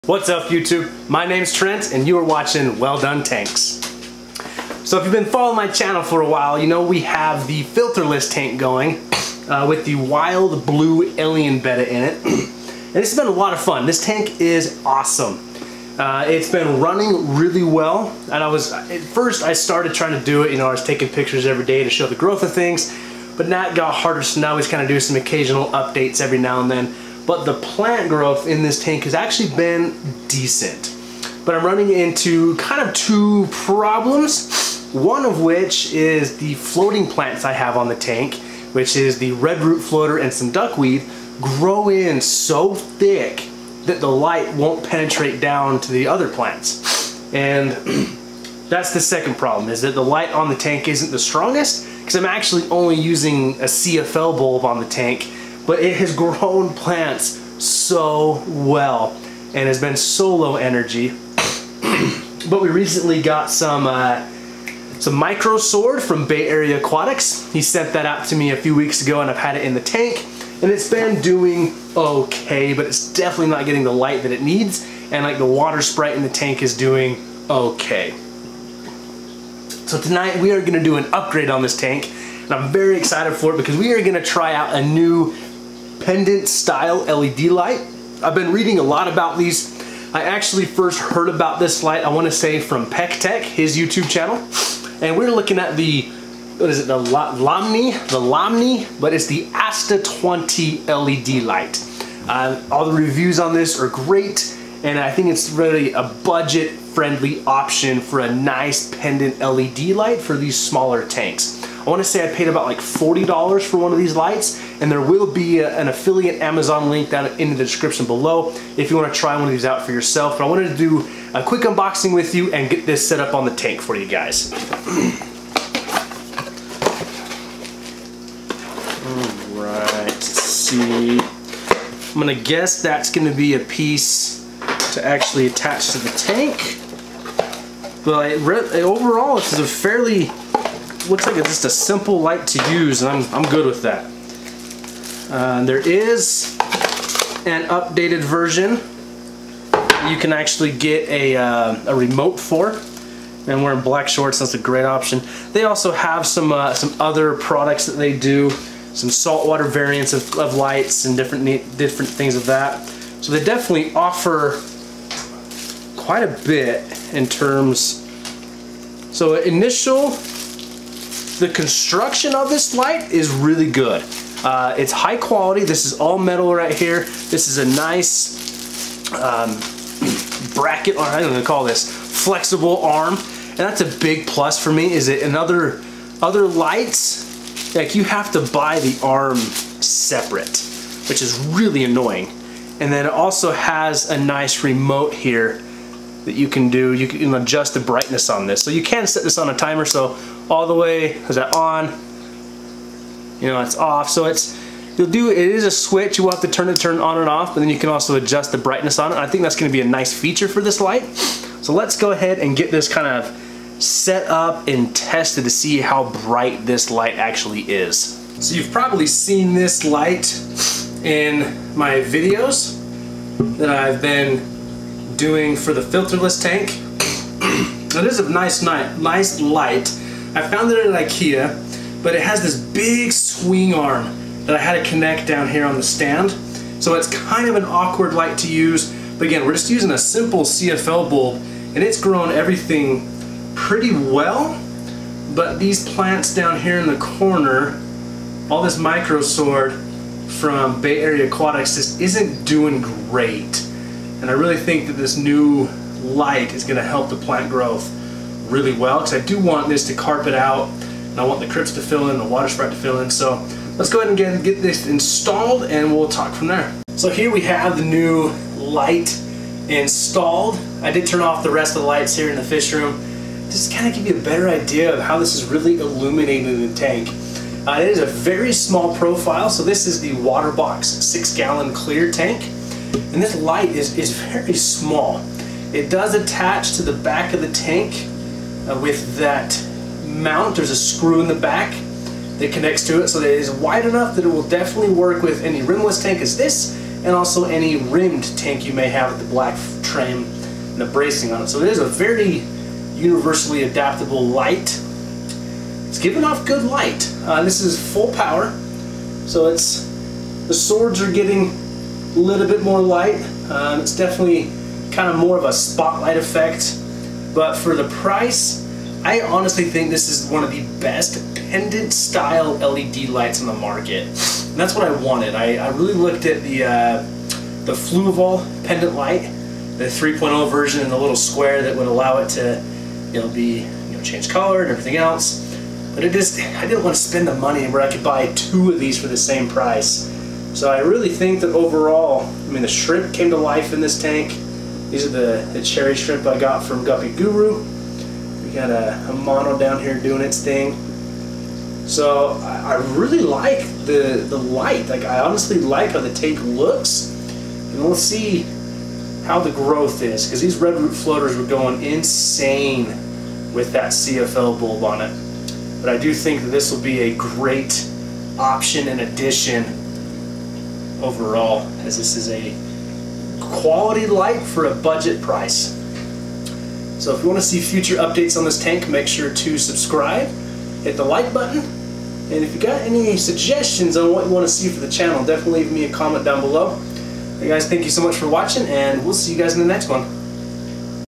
Asta 20 LED Light Review
Today we look at a budget friendly led light option.  The Asta 20 is a great option!

Asta 20 Led Light

Asta 20 Led Light (UK Link)